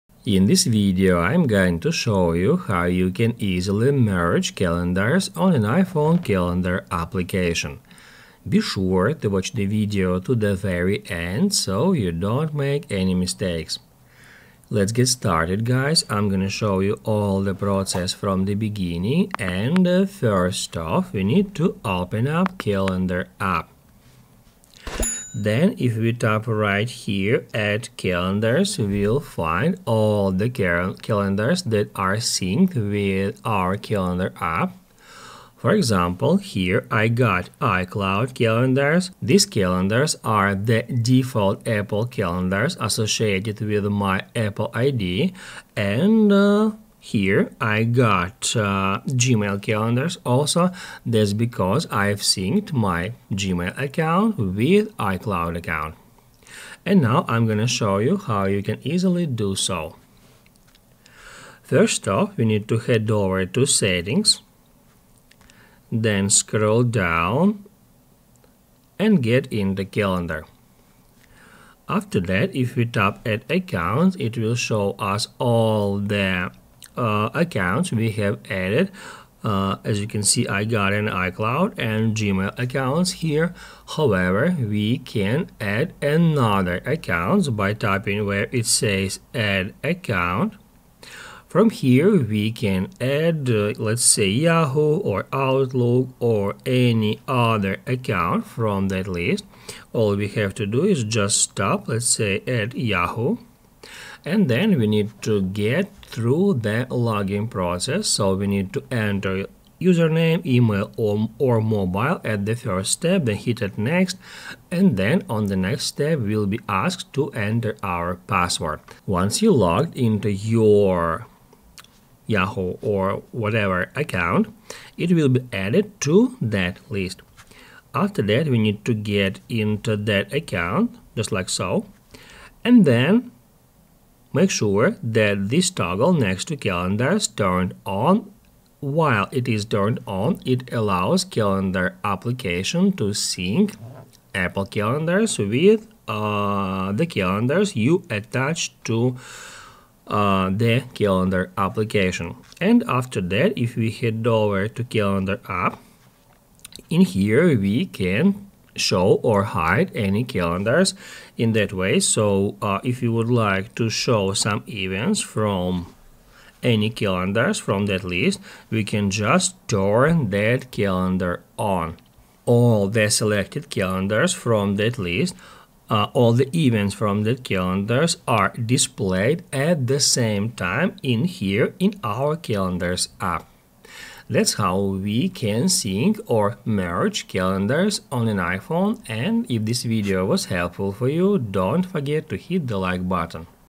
How to Merge Calendars on iPhone
If you are looking for a video about how to merge calendars on iphone, here it is! In this video I will show you how to share calendar on iphone. Be sure to watch the video to the very end.